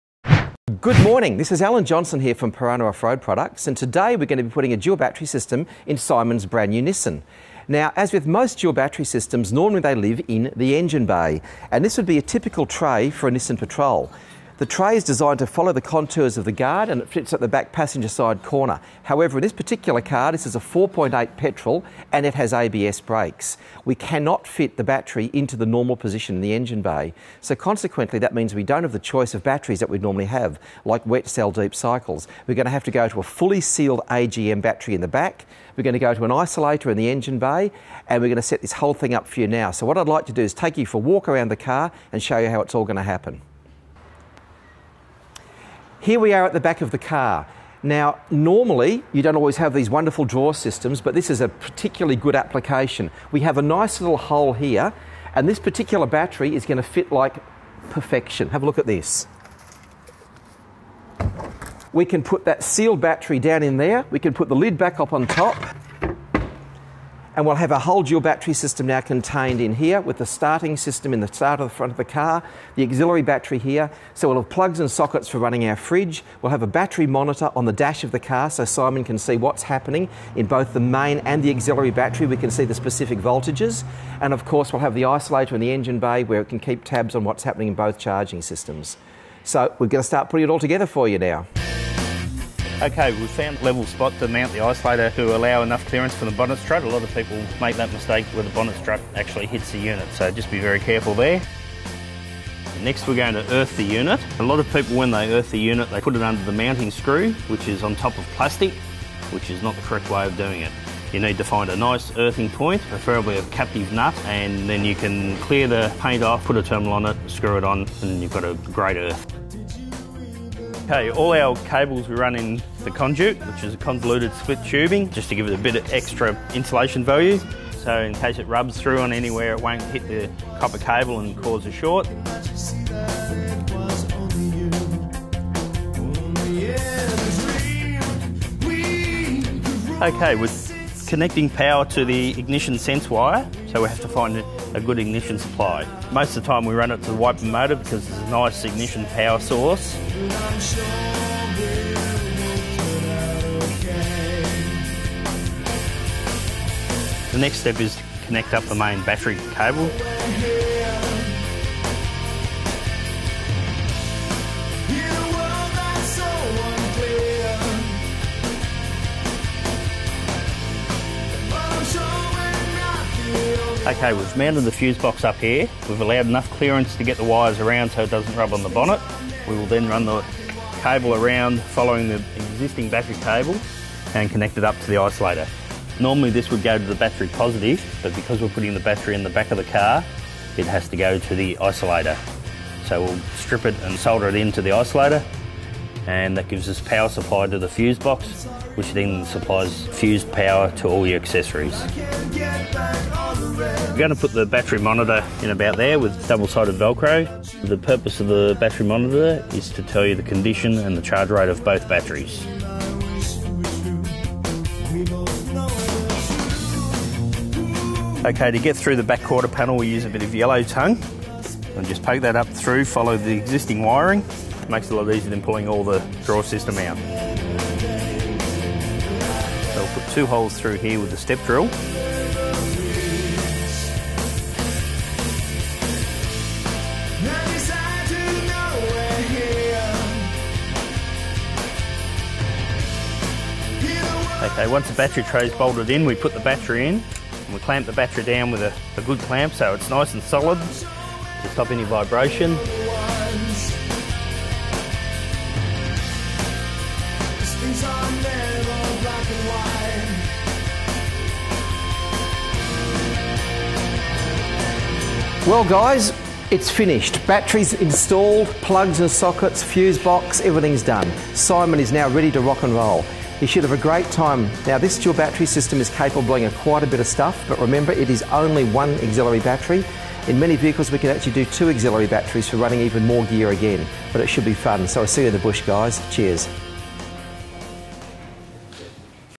GU Patrol Dual Battery System Installation
Alan from Piranha Offroad does his magic on Simon and Miranda's 4wd TV GU Patrol. ABS means that there is no room under the bonnet for the second battery but the Black Widow draw system is the ideal place for a sealed AGM Exide Orbital.